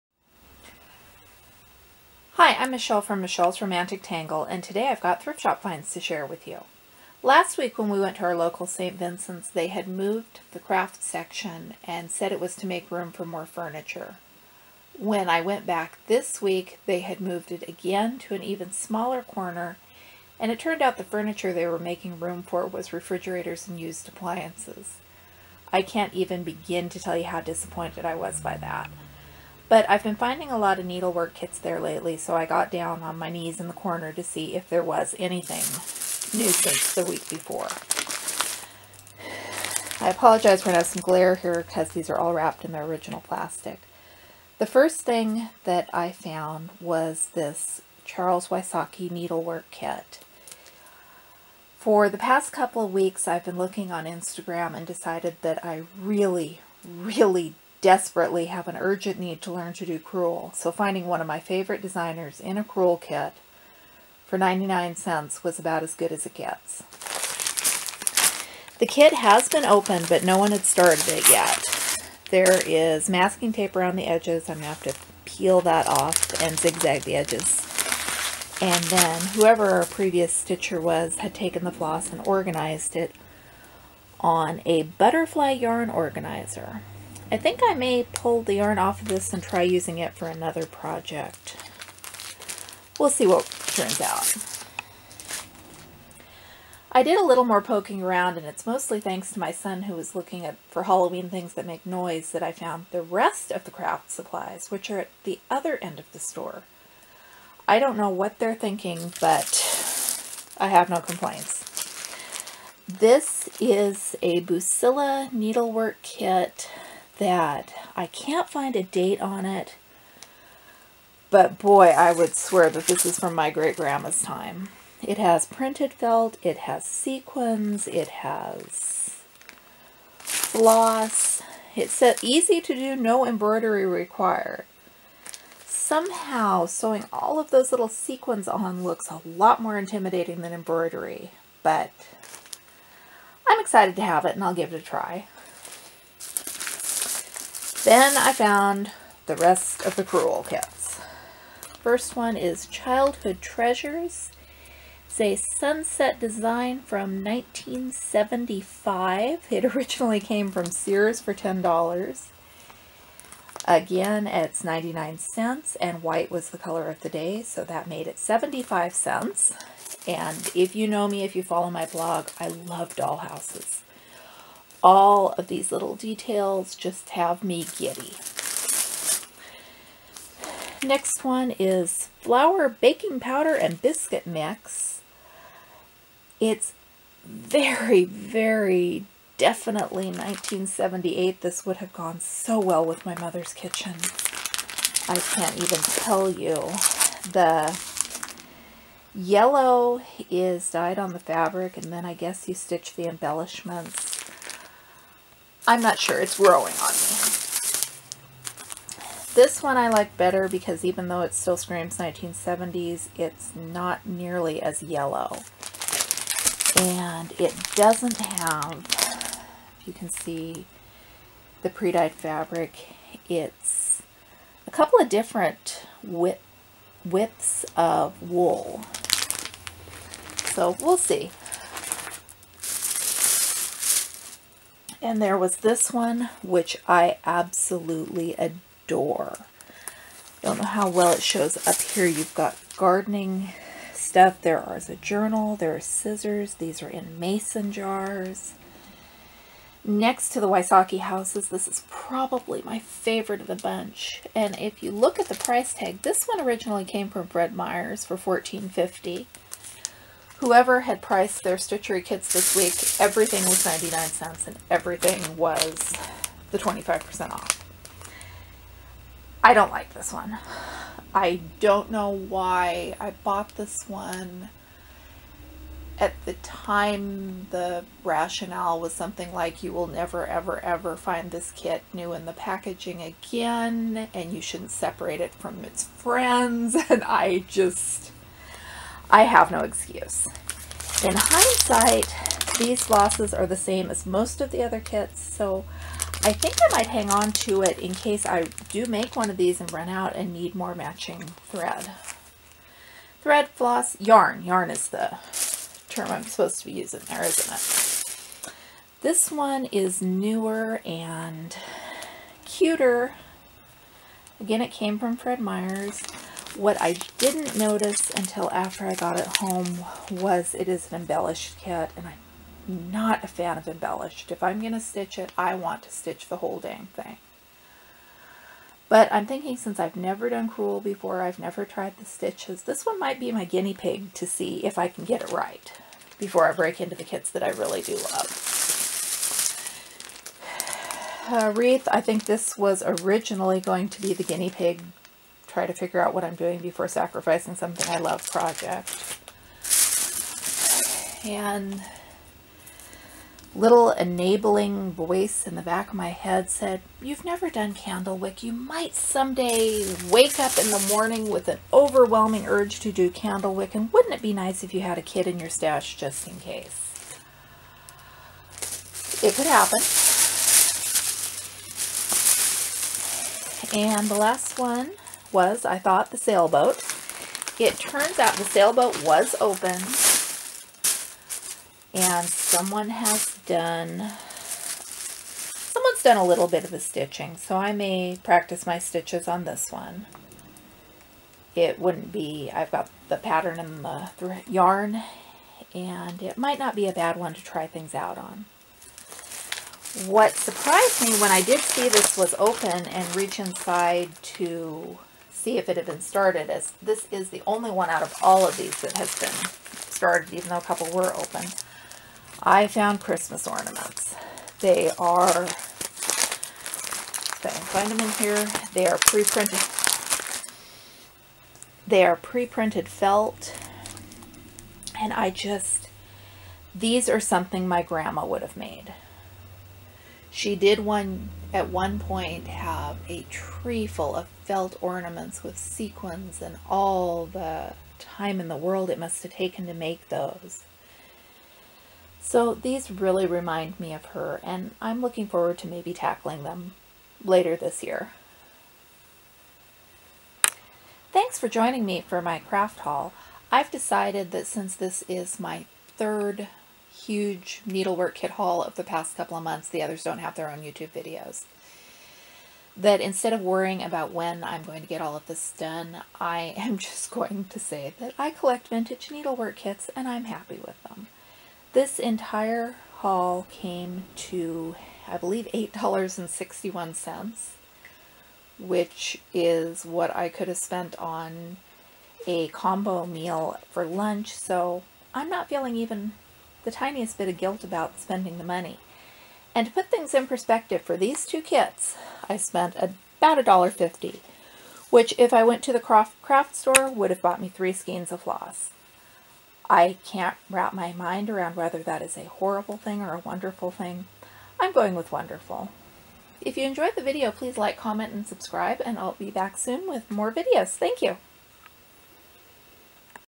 Thrift Store Craft Haul -- All the 70s Needlework Kits! (October 2017)
I don't know what is going on at this thrift store -- if a family is slowly donating someone's huge collection or if there's more than one mystery stitcher...but they keep having huge hauls of needlework stuff and I keep bringing it home with me.